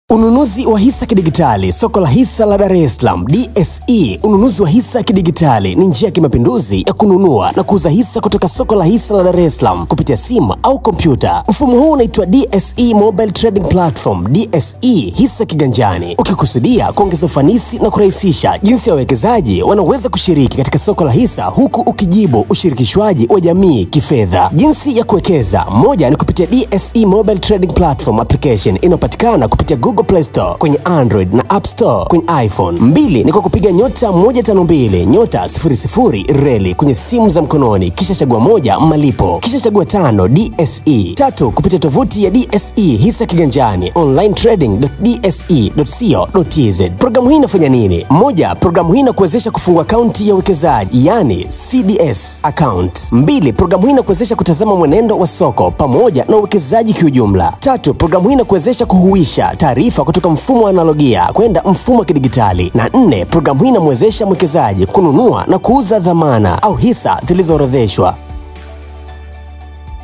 DSE MOBILE TRADING PLATFORM / DSE HISA KIGANJANI.. WEKEZA KIDIGITALI
DSE Mobile Trading Platform / DSE Hisa Kiganjani ni mfumo unaowezesha kufungua akaunti ya uwekezaji "CSD Account", kuangalia mwenendo wa Hisa katika Soko, kuhuwisha taarifa kutoka analogia kwenda digitali, kununua na kuuza dhamana (hisa / hatifungani) zilizoorodheshwa